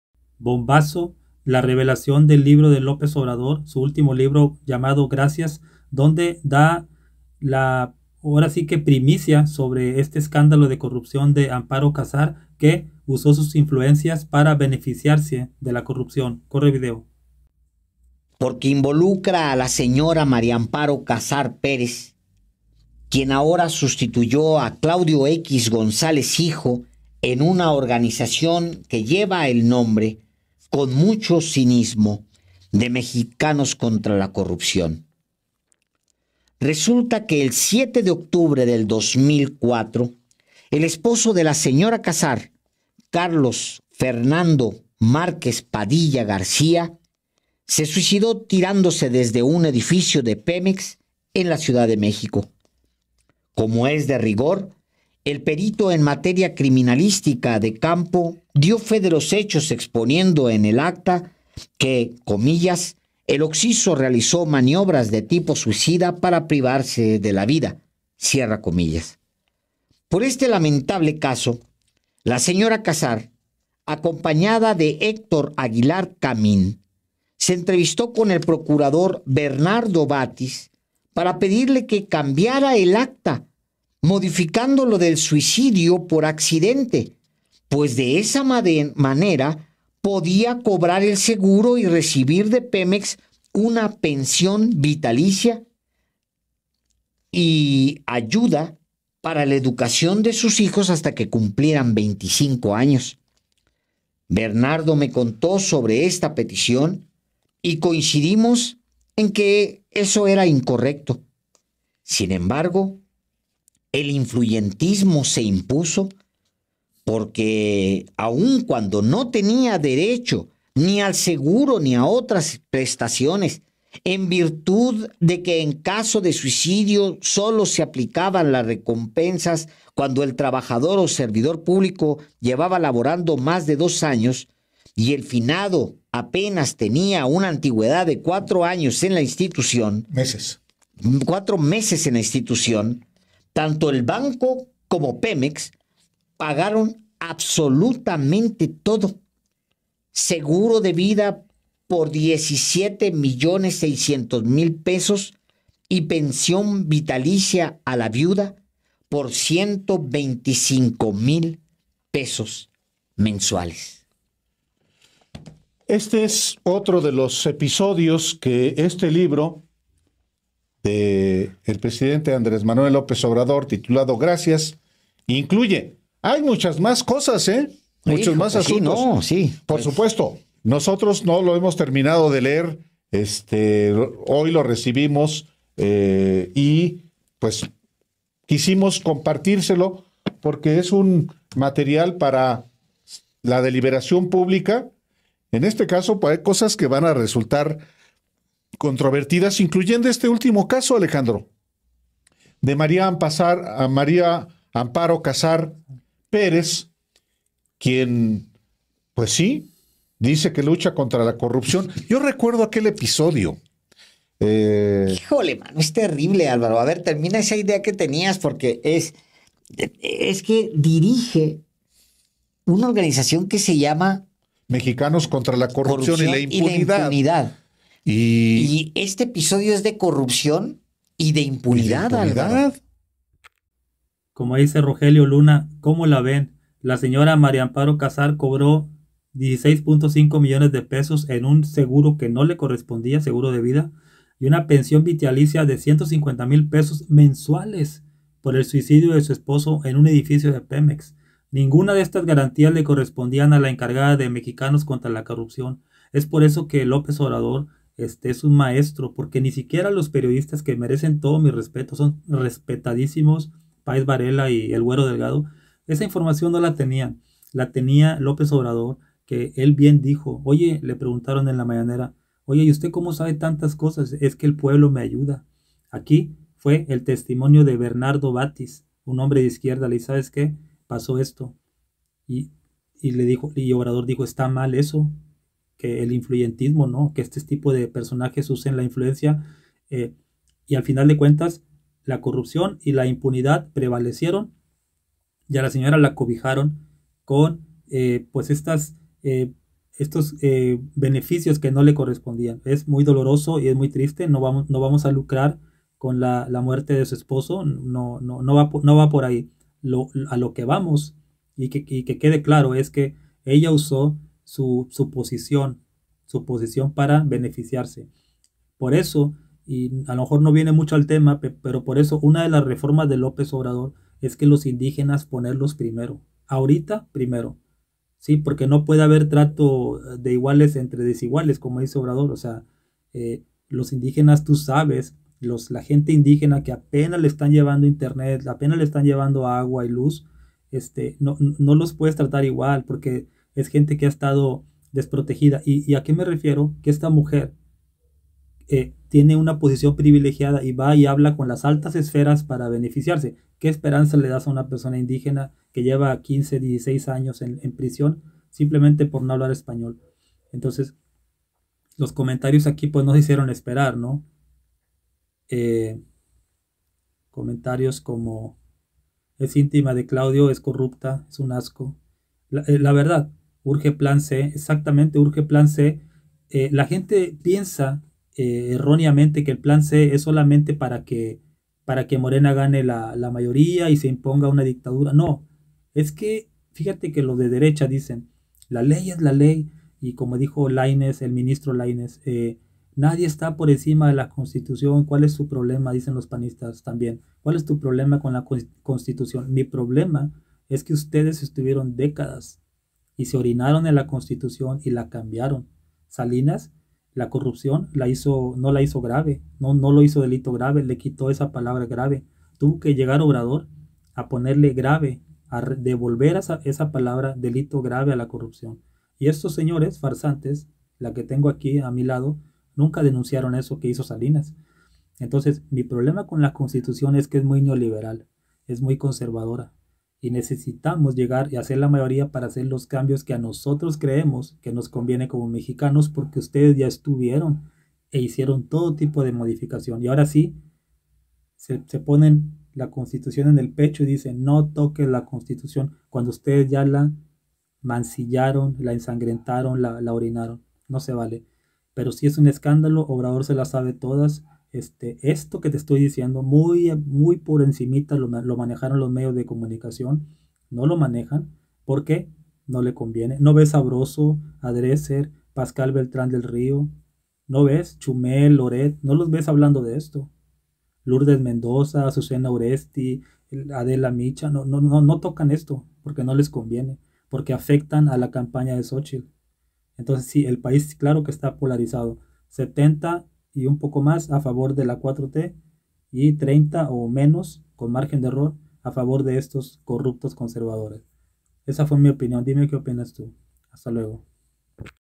¡NOTICIÓN!, AMLO revela escándalo de Amparo Casar
López obrador revela en su último libro llamado "gracias" que amparo casar, presidente de la asociación "mexicanos contra la corrupción y la impunidad" y amiga de Claudio X gonzalez utilizó sus influencias con su amigo Héctor aguilar camín para cobrar beneficios de pemex que no le correspondían tras el fallecimiento de su esposo.